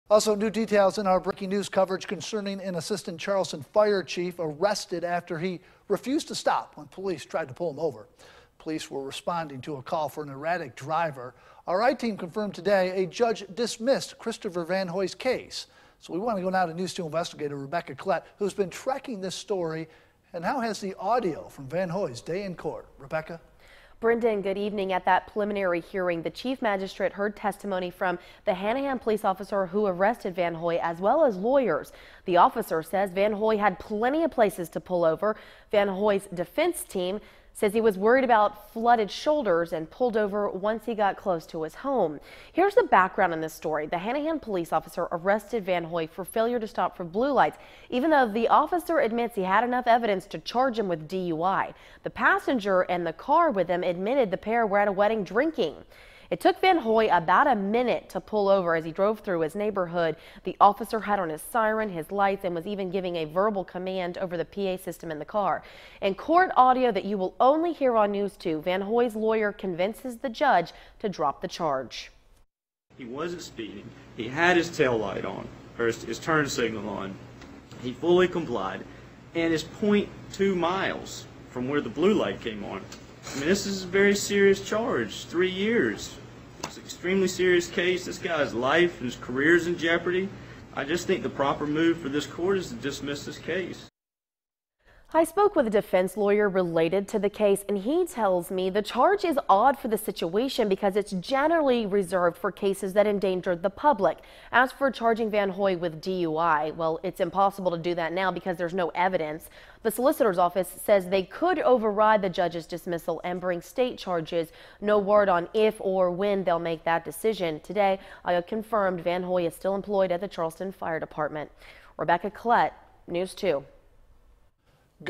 I-Team: Assistant Fire Chief’s case dismissed
The News 2 I-Team confirmed Wednesday the case against Christopher Vanhoy, the Charleston Assistance Fire Chief, was dismissed at a preliminary hearing.